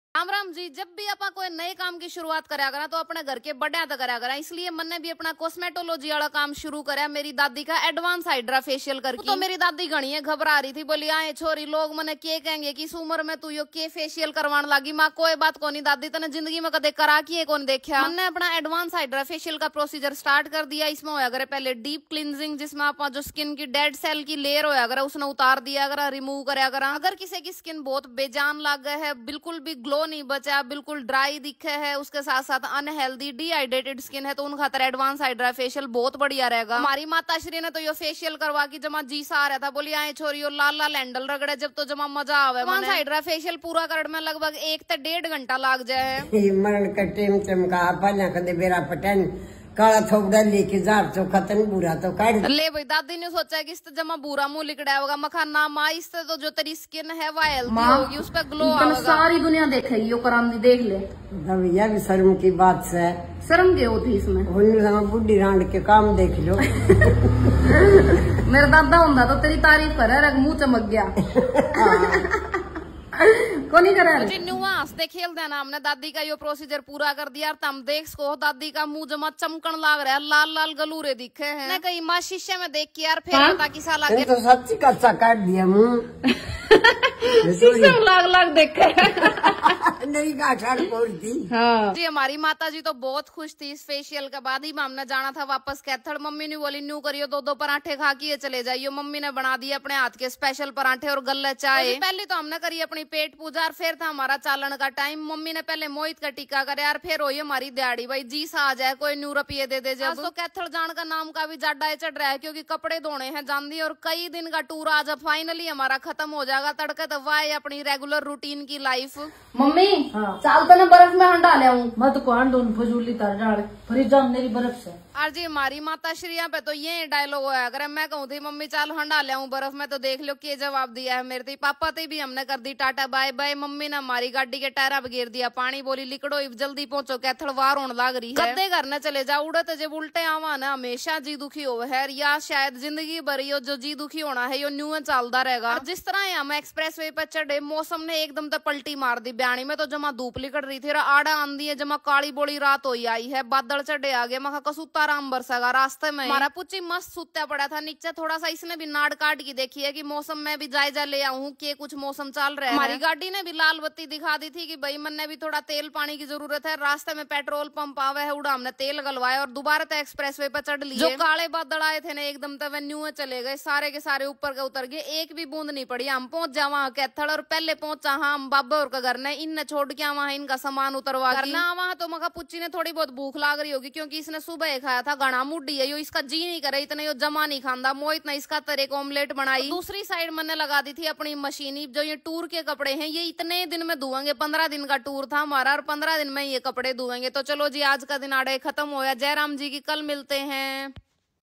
hmari pyari daadi ne krwaya jindagi me phli baar advance hydra facial
dekhiye ksa rha daadi ka reaction
lambi chutti k baad chal diye kaithal vapas